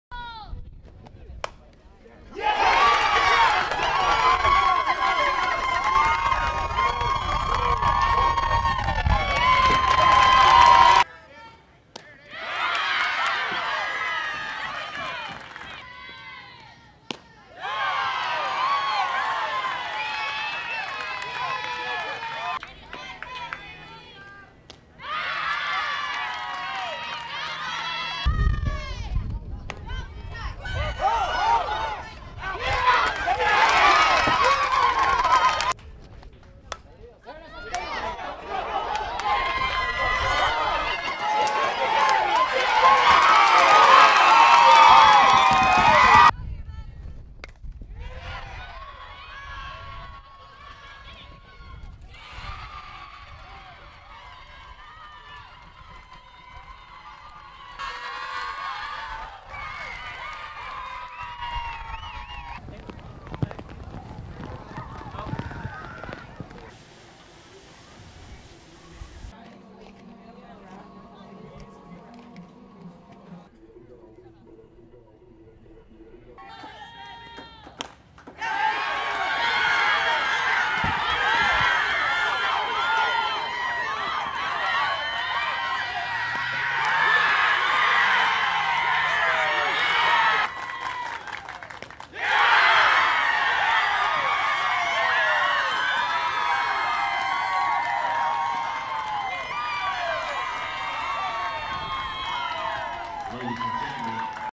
Big South Conference Softball Tournament - Game 13 - Longwood vs. Coastal Carolina
Longwood beats Coastal Carolina 5-2 to advance to the Championship game of the Big South Conference Softball Tournament.